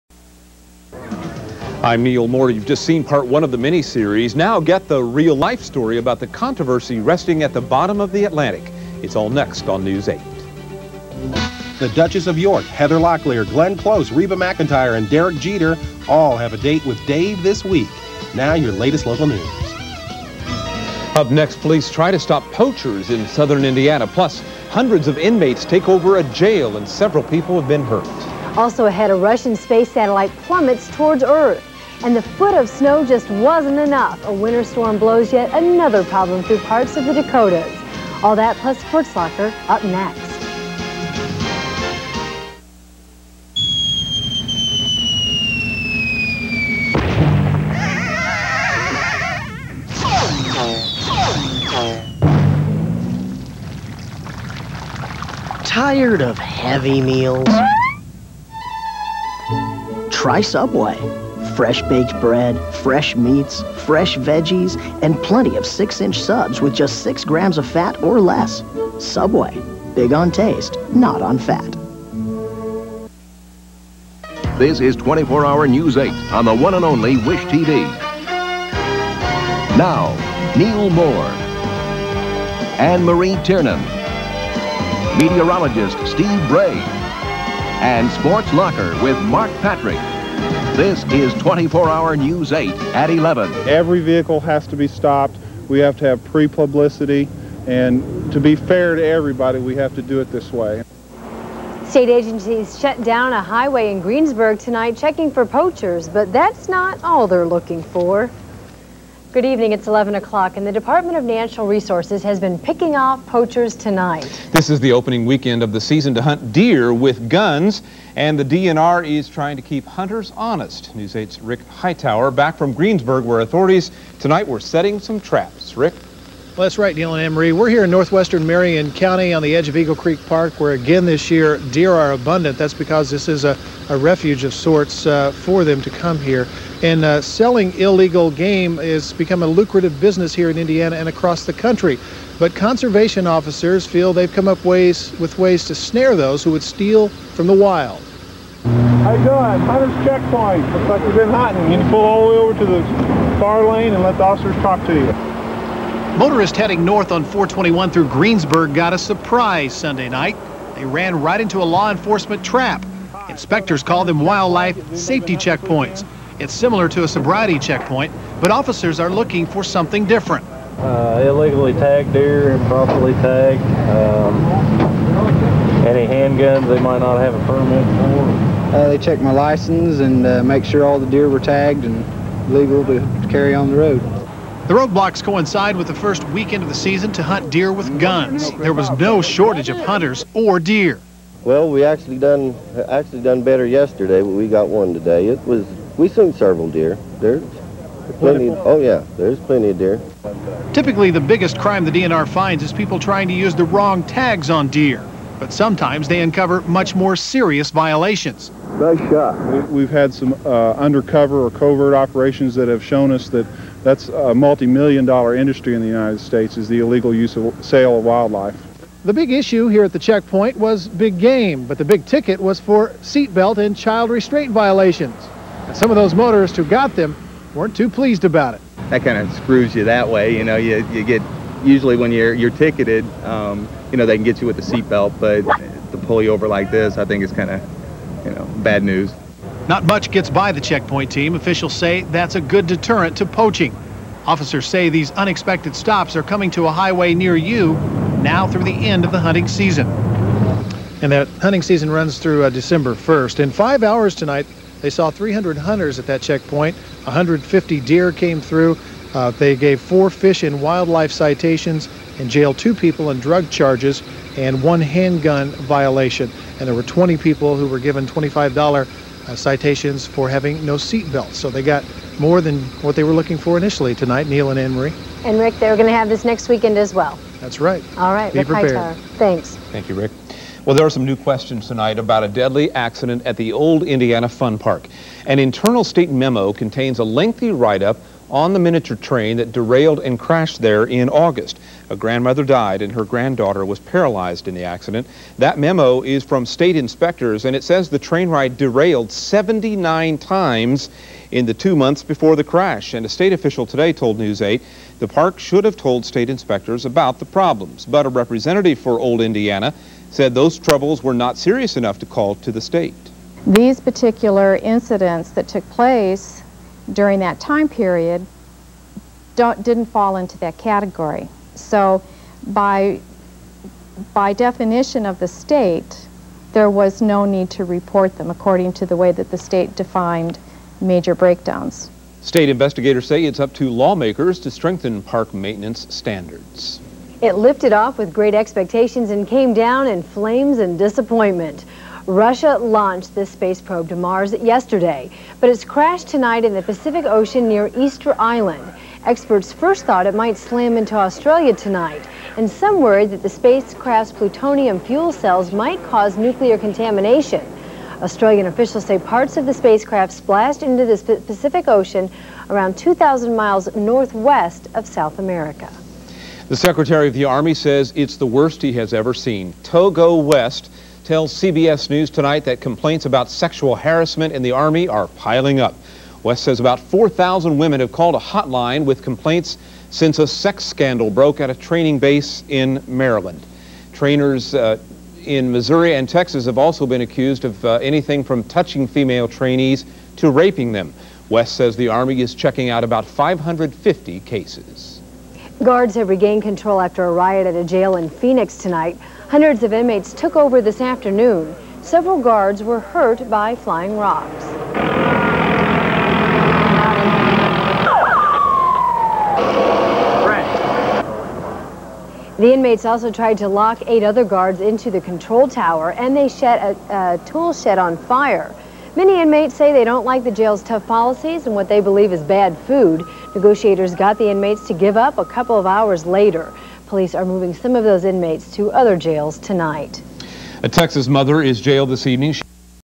November 17, 1996 - Indianapolis 11PM Newscast (Partial)
Anne Marie Tiernon and Neal Moore are the anchors and reporter Rick Hightower is seen.  Aired following part one the miniseries 'Titanic'.